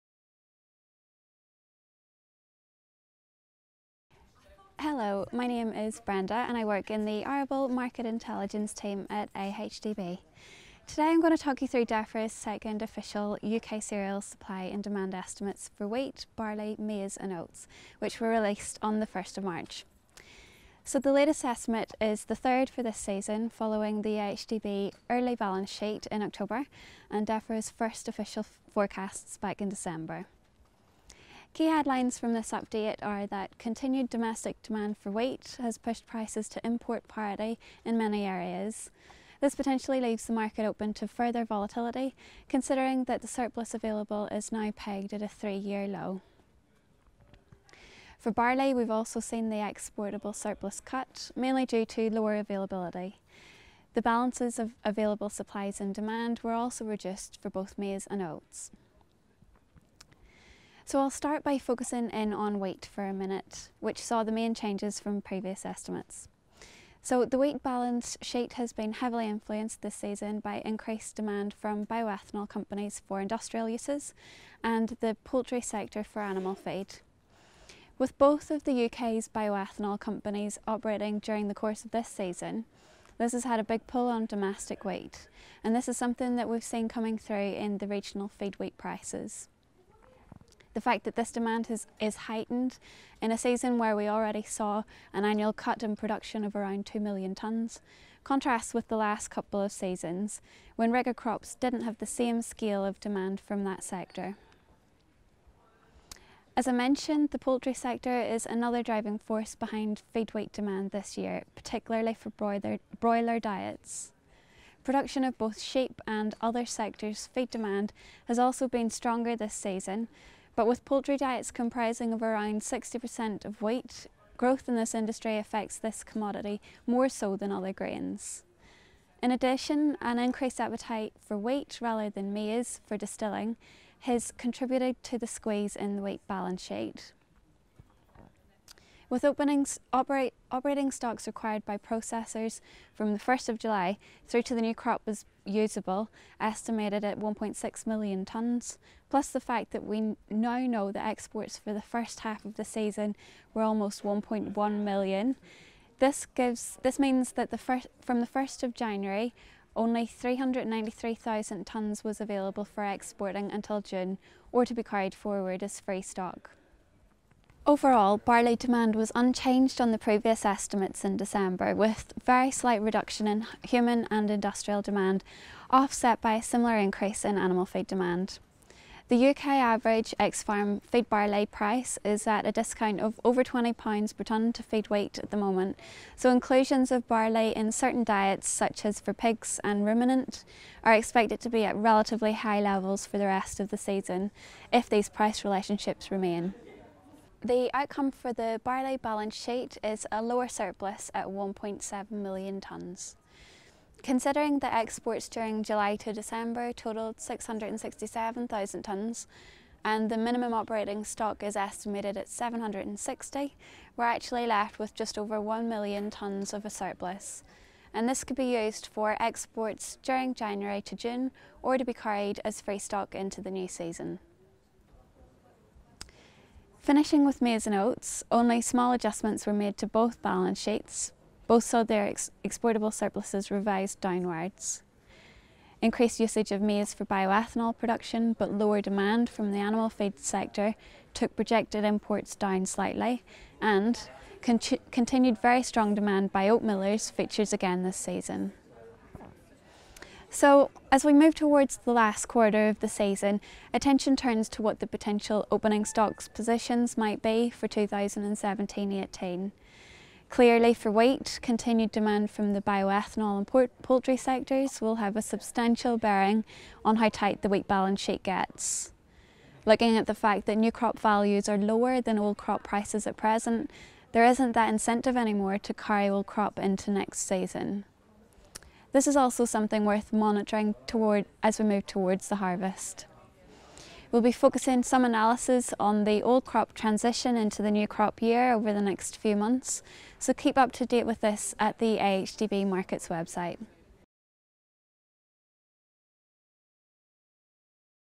UK cereals supply & demand update
Brenda Mullan, from AHDB Cereals & Oilseeds, provides an insight into the latest updates to Defra’s UK cereals balance sheets for 2016/17, including some of the assumptions behind the forecasts.